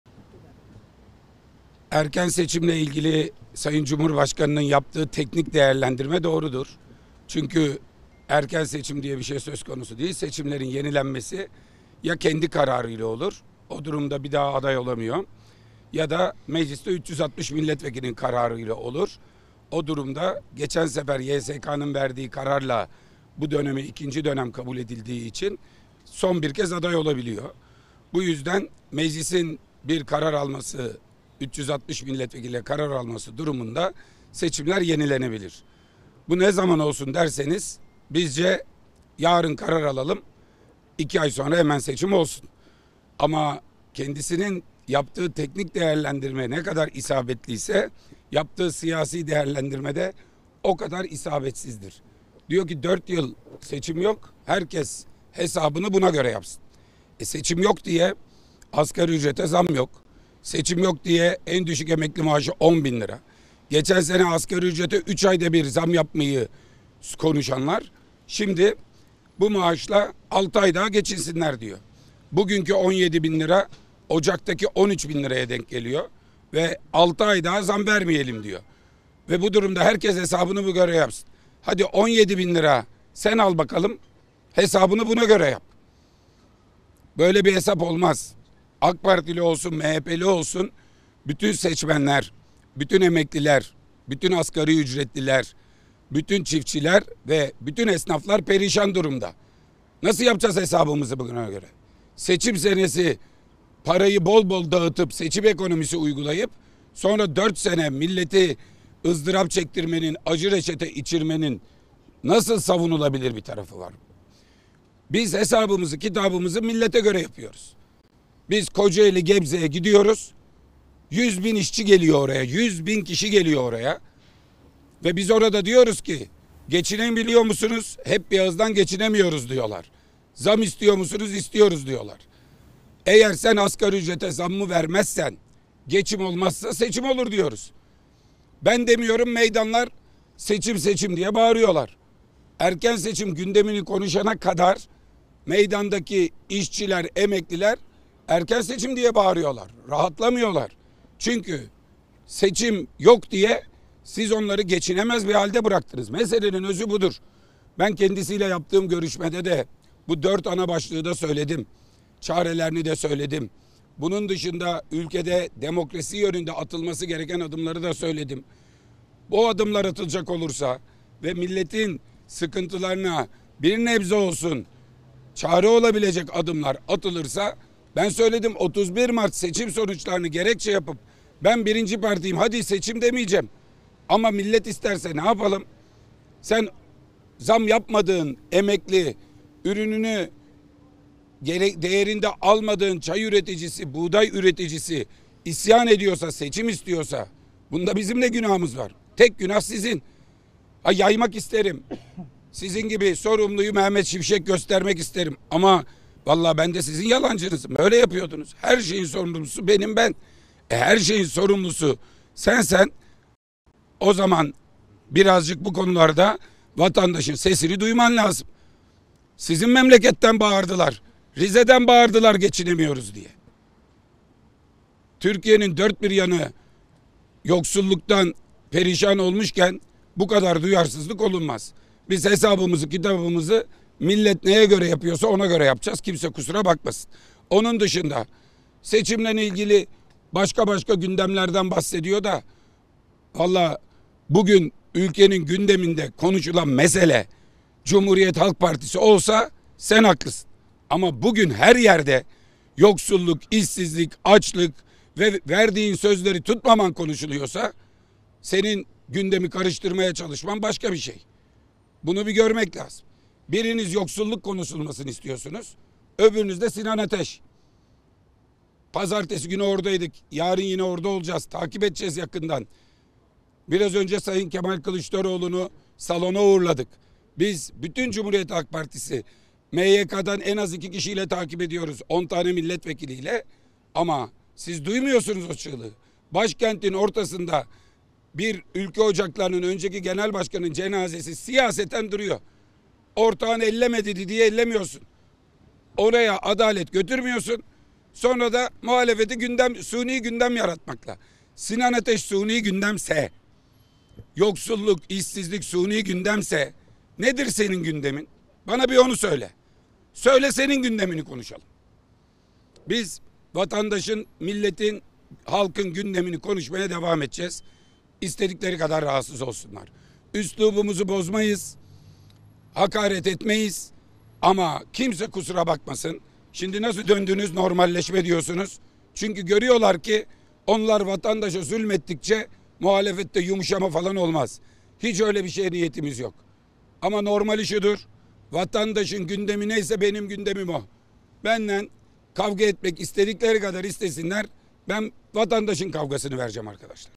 SONDAKİKA Özgür Özel'den Erdoğan'a flaş 'erken seçim' yanıtı: Seçim yok diye zam yoksa...
Özgür Özel'den Erdoğan'a flaş 'erken seçim' yanıtı: Seçim yok diye zam yoksa...